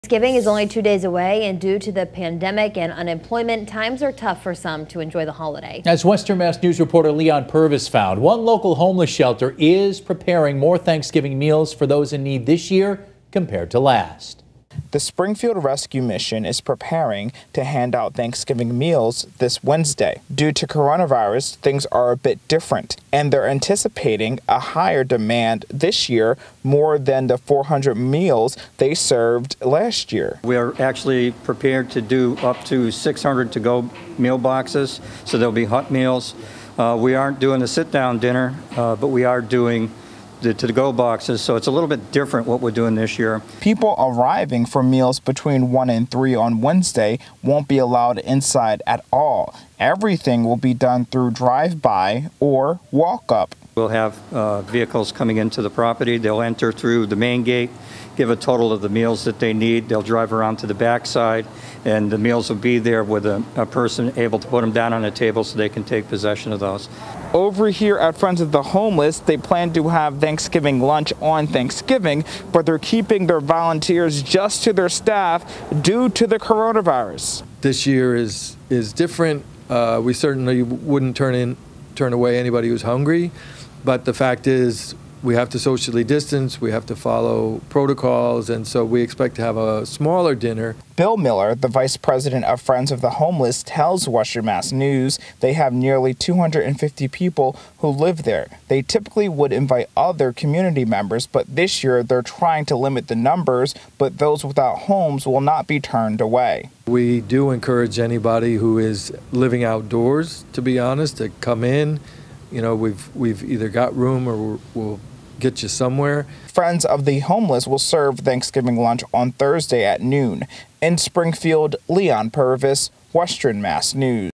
Local groups adjusting their Thanksgiving meals due to the pandemic
One local homeless shelter is preparing more Thanksgiving meals for those in need this year compared to last.